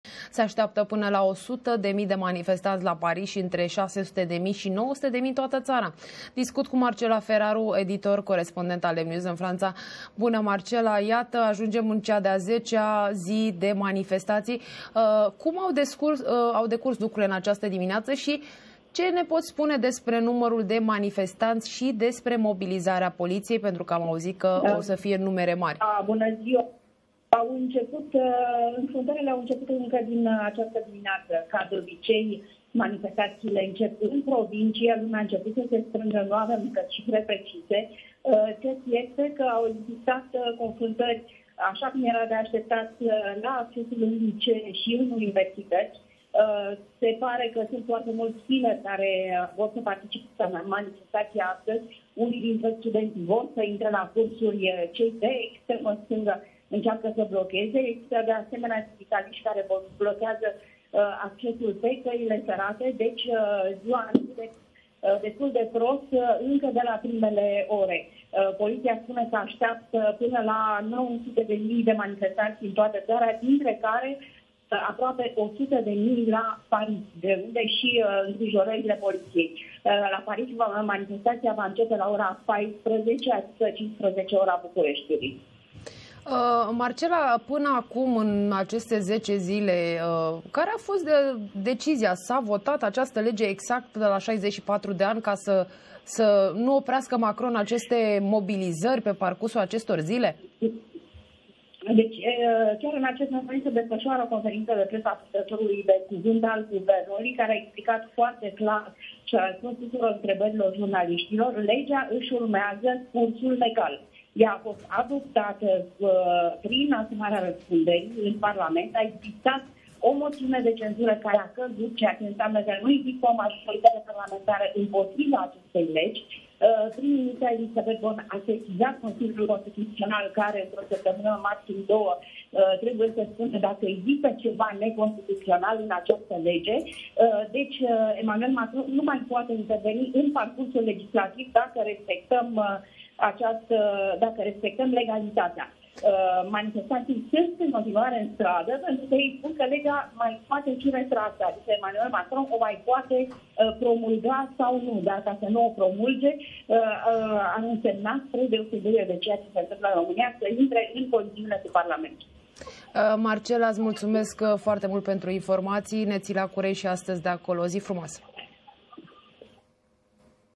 Vezi ACUM: Protestele continuă în Franța. Astăzi sunt așteptați 900.000 de oameni în stradă
➥ Protestele față de reforma pensiilor continuă în Franța.
➥ Astăzi sunt așteptați 900.000 de oameni în stradă.
➥ MAE a emis o atenționare de călătorie pentru românii care călătoresc în Franța.